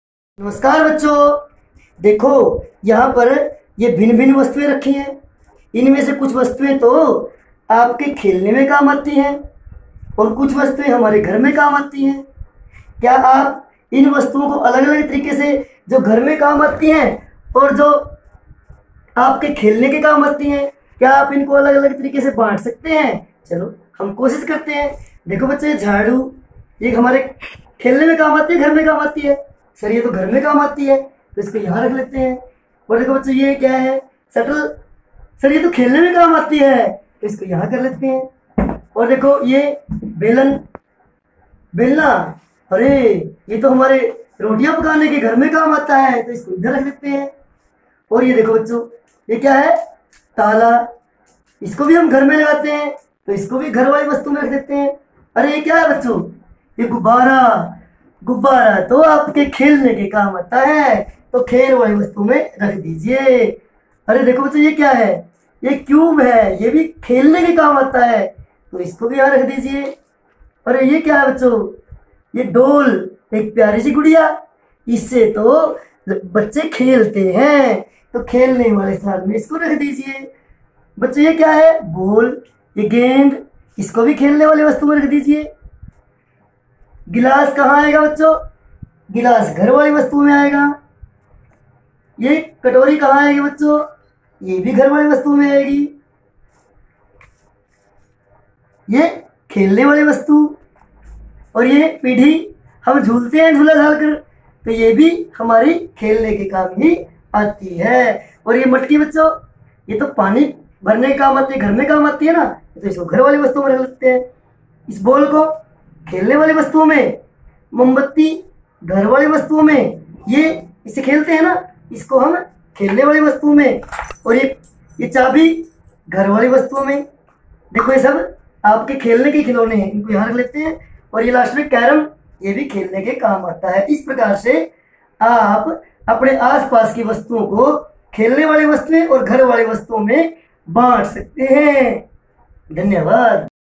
NCERT MATH CLASS -1 गेंद और डिब्बा , CLASSIFY THINGS AS KITCHEN AND PLAYING THINGS
CLASS-1#
MATH SCHOOL WITH MAHAVIR SIR#
NCERT MATH#

इस विडियो को देखकर बच्चे वस्तुओं में से खेलसकने वाली वस्तुएँ तथा रसोई की वस्तुओं अलग अलग कर सकेंगें